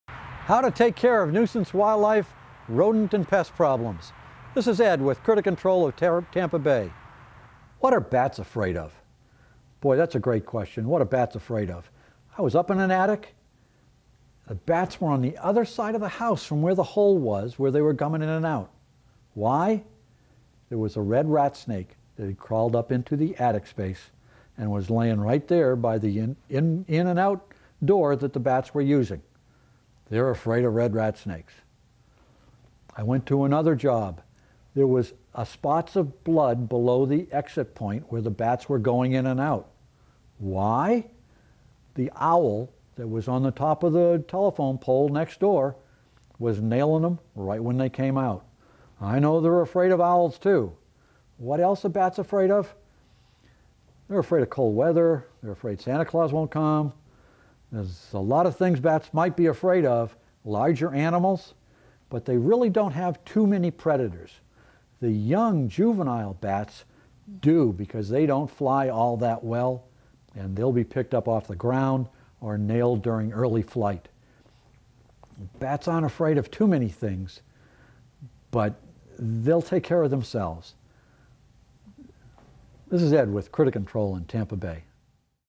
Pest Control : What Are Bats Afraid Of?
Bats are generally afraid of their predators, which include snakes and owls, but little else is known of their fears. Learn about bats' fear patterns with information from a wildlife control specialist in this free video on pest control.